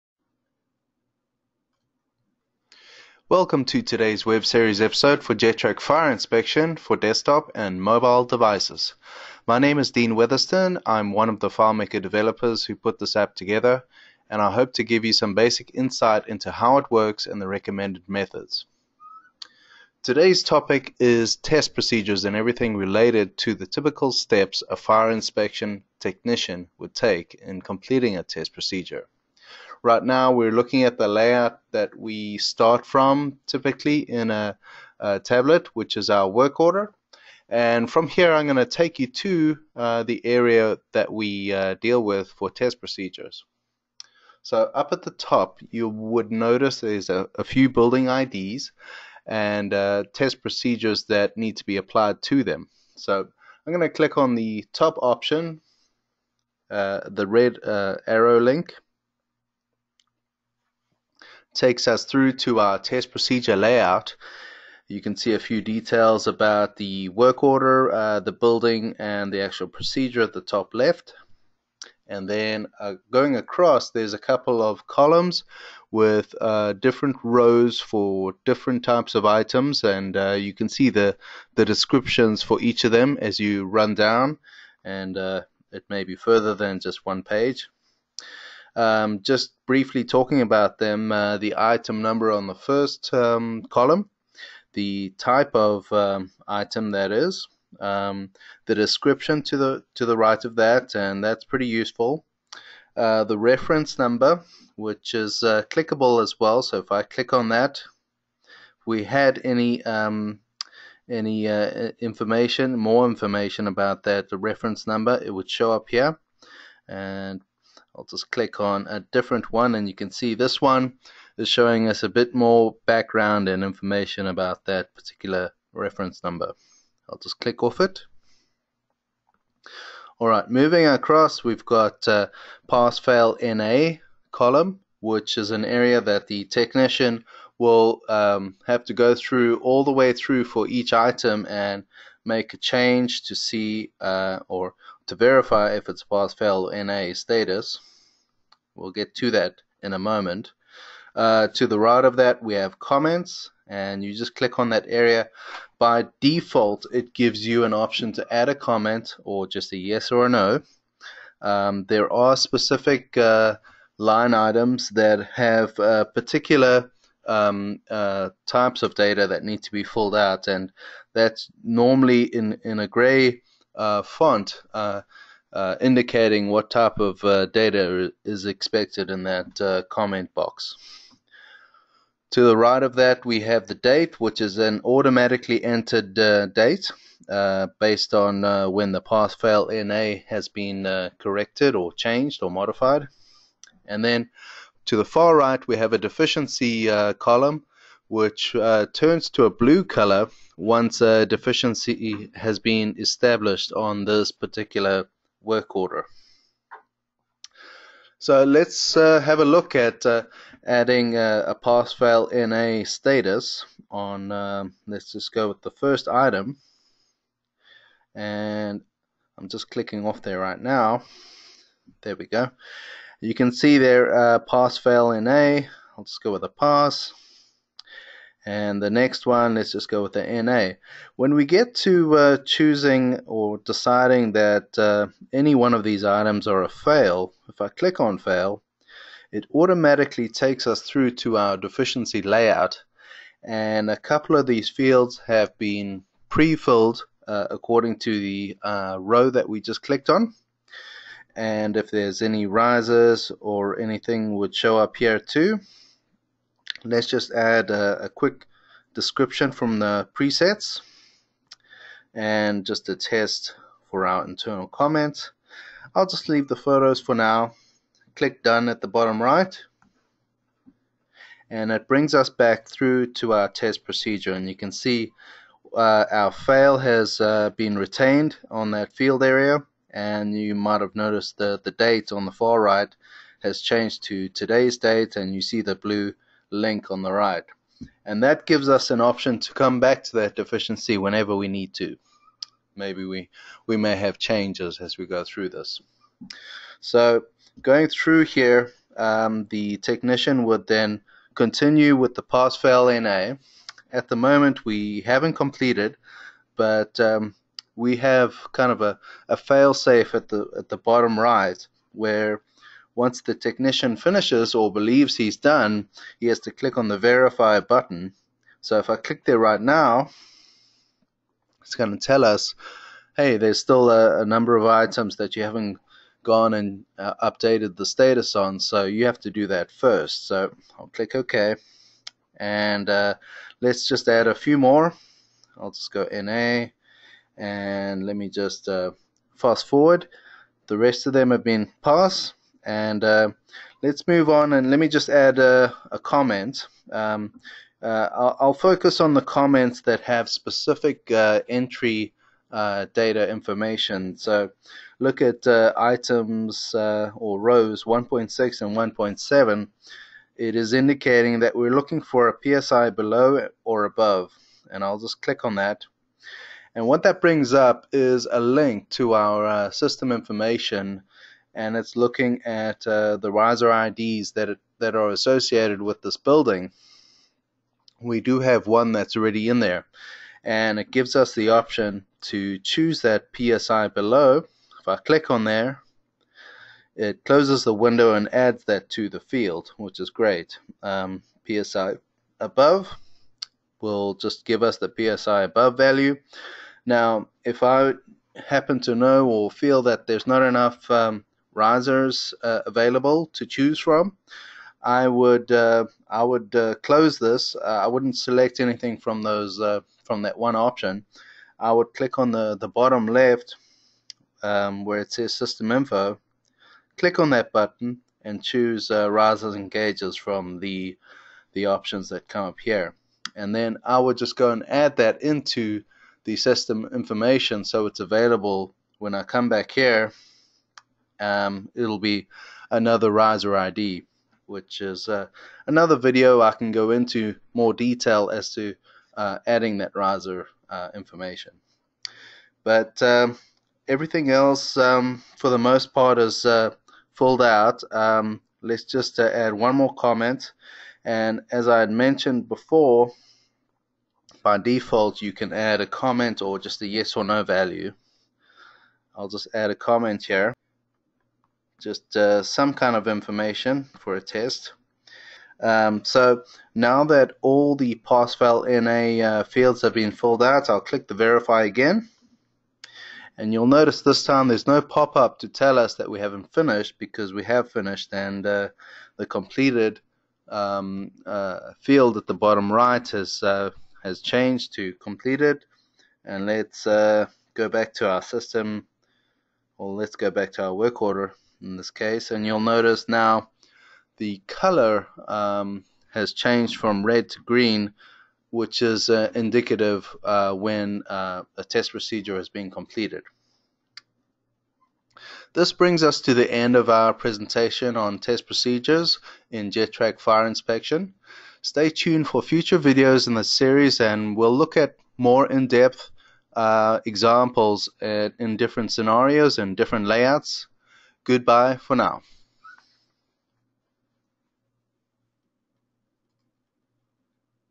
JetTrac Fire Inspection - Part 6 - Test Procedure on iPad
Running through some of the typical steps a technician will make to complete a test procedure and some of the primary options to be made on the way.